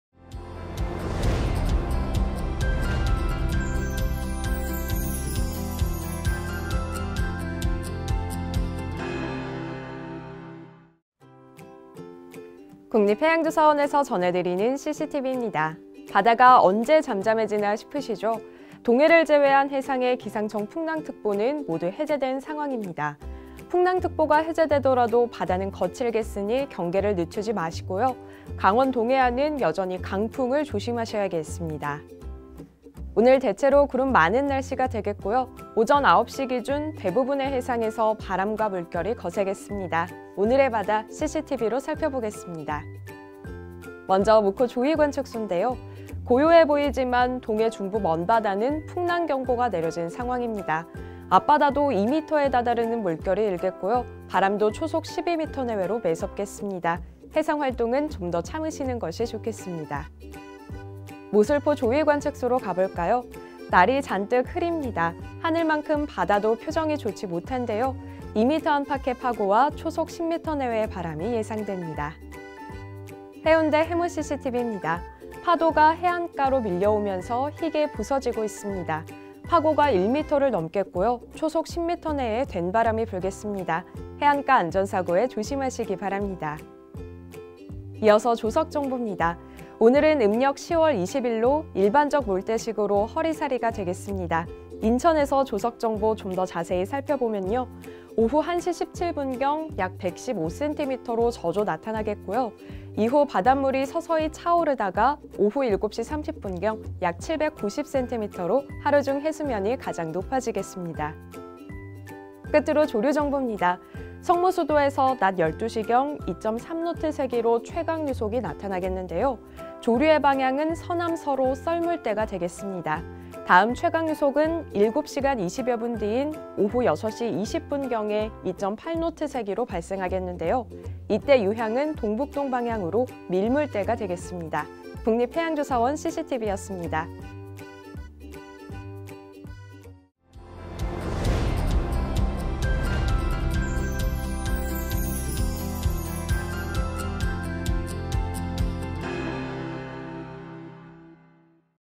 [See Sea TV] 2021년 11월 24일 대부분 해상 거친 파도, 안전사고 주의!/ 해양예보 On바다
▶ 기사 원문 :
국립해양조사원에서 전해드리는 See Sea TV입니다.
바다가 언제 잠잠해지나 싶으시죠.
동해를 제외한 해상의 기상청 풍랑특보는 모두 해제된 상황입니다.
풍랑특보가 해제되더라도 바다는 거칠겠으니 경계를 늦추지 마시고요.
강원동해안은 여전히 강풍을 조심하셔야겠습니다.

오늘 대체로 구름 많은 날씨가 되겠고요.
오전 9시 기준 대부분의 해상에서 바람과 물결이 거세겠습니다.
오늘의 바다, CCTV로 살펴보겠습니다.

먼저 묵호 조위관측소인데요.
고요해 보이지만 동해 중부 먼바다는 풍랑경보가 내려진 상황입니다.
앞바다도 2m에 다다르는 물결이 일겠고요.
바람도 초속 12m 내외로 매섭겠습니다.
해상활동은 좀 더 참으시는 것이 좋겠습니다.

모슬포 조위관측소로 가볼까요.
날이 잔뜩 흐립니다.
하늘만큼 바다도 표정이 좋지 못한데요.
2m 안팎의 파고와 초속 10m 내외의 바람이 예상됩니다.

해운대 해무 CCTV입니다.
파도가 해안가로 밀려오면서 희게 부서지고 있습니다.
파고가 1m를 넘겠고요.
초속 10m 내외의 된바람이 불겠습니다.
해안가 안전사고에 조심하시기 바랍니다.

이어서 조석정보입니다.
오늘은 음력 10월 20일로 일반적 물때식으로 허리사리가 되겠습니다.
인천에서 조석정보 좀 더 자세히 살펴보면요.
오후 1시 17분경 약 115cm로 저조 나타나겠고요.
이후 바닷물이 서서히 차오르다가 오후 7시 30분경 약 790cm로
하루 중 해수면이 가장 높아지겠습니다.

끝으로 조류정보입니다.
석모수도에서 낮 12시경 2.3노트 세기로 최강유속이 나타나겠는데요.
조류의 방향은 서남서로 썰물때가 되겠습니다.
다음 최강유속은 7시간 20여분 뒤인 오후 6시 20분경에 2.8노트 세기로 발생하겠는데요.
이때 유향은 동북동방향으로 밀물때가 되겠습니다.
국립해양조사원 see sea TV였습니다.